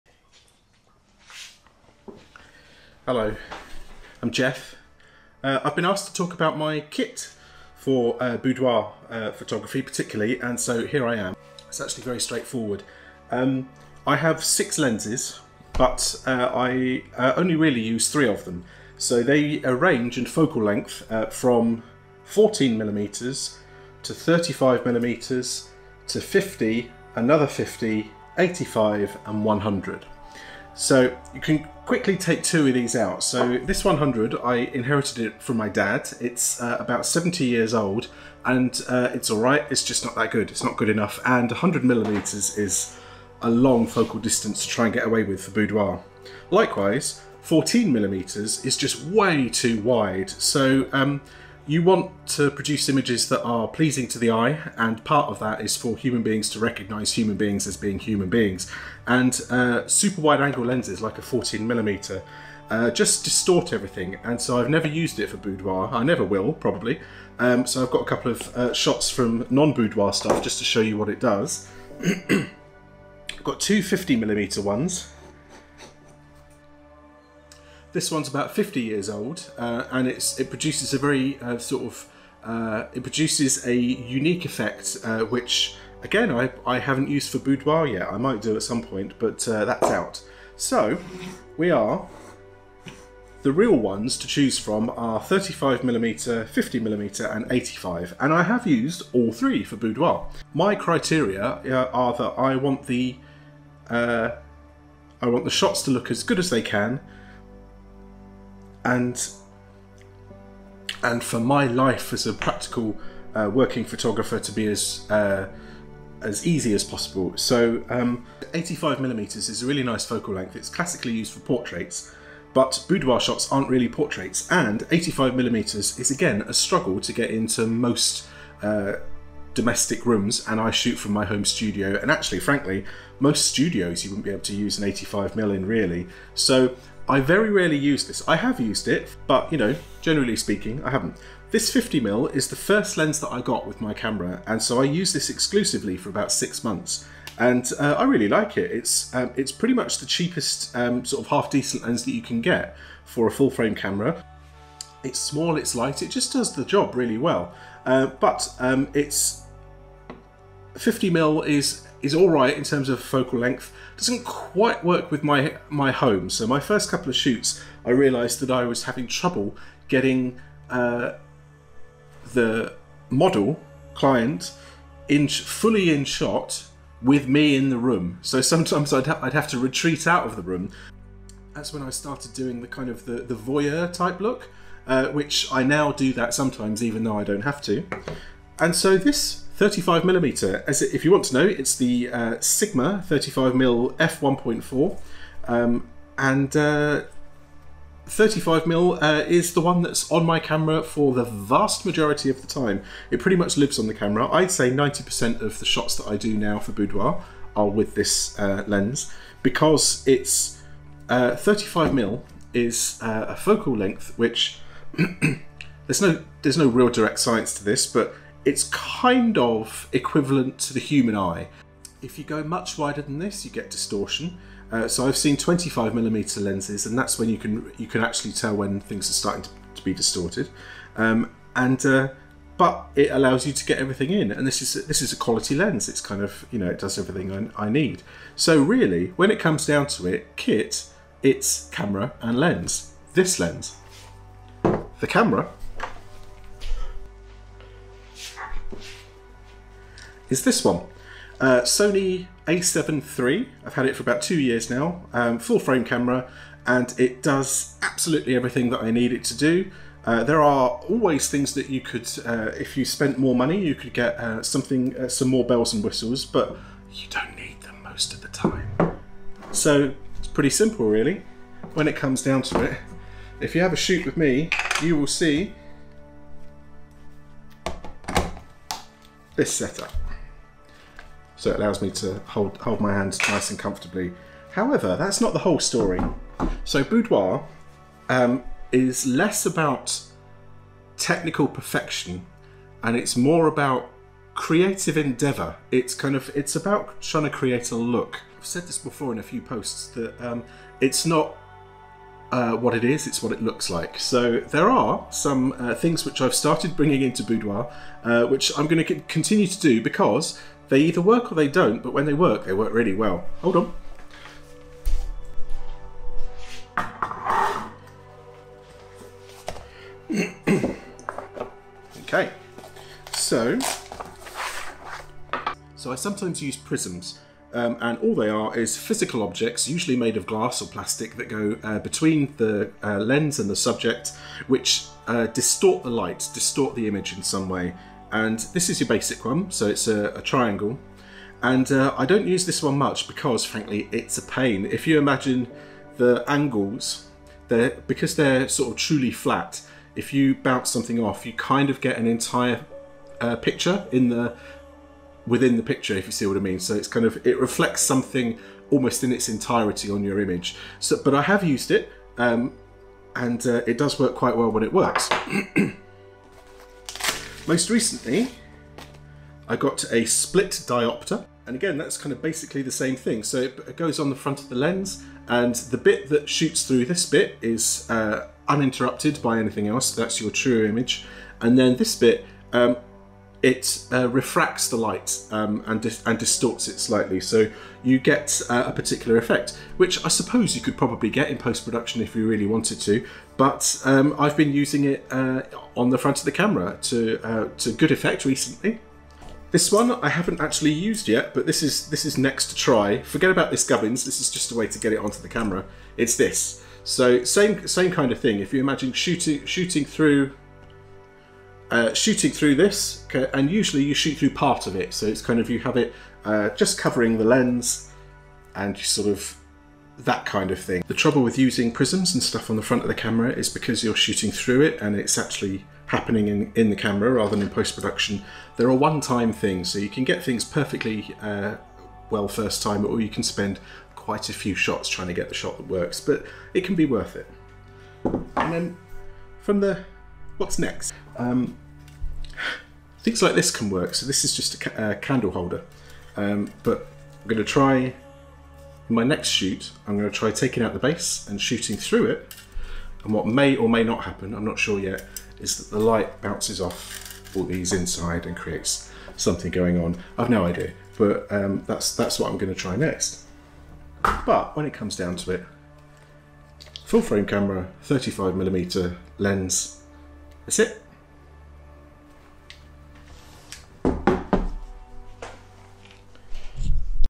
My Kit for Boudoir Photography
I've never been about the kit, but obviously a photographer's gotta have some.  Here's my rundown of my boudoir essential kit.  Maybe not what you'd expect.